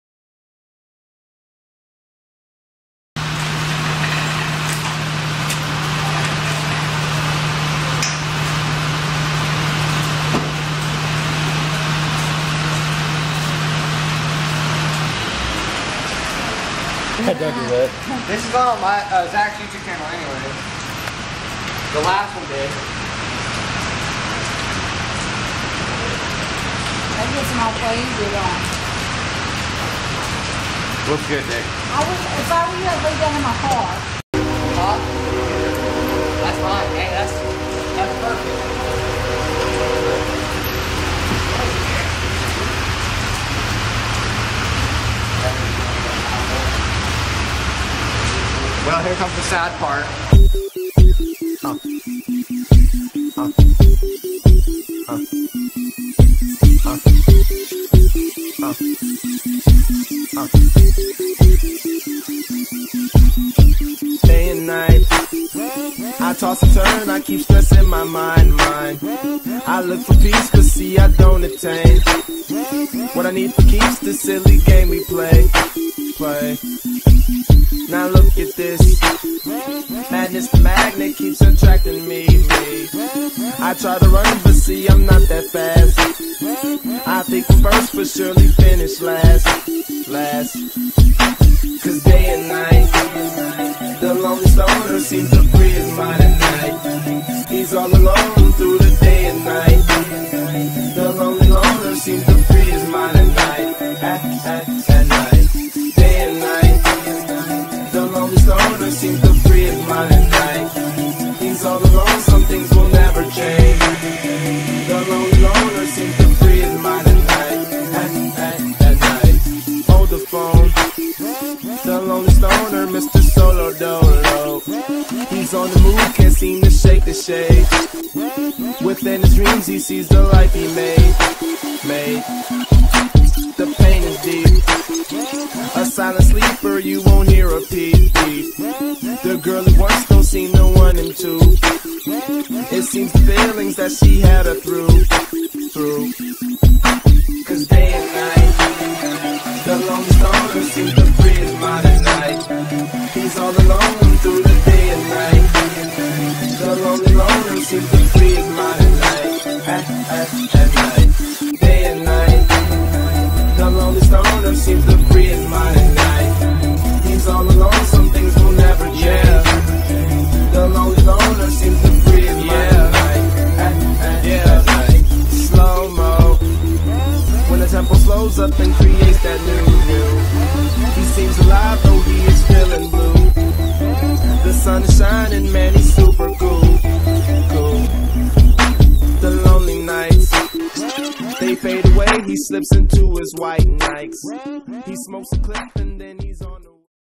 Honda Accord 03 wide body
Have a small project putting 2 inch wheel spacers and fender flared on a 2003 honda accord k series xxr wheels on ground control coils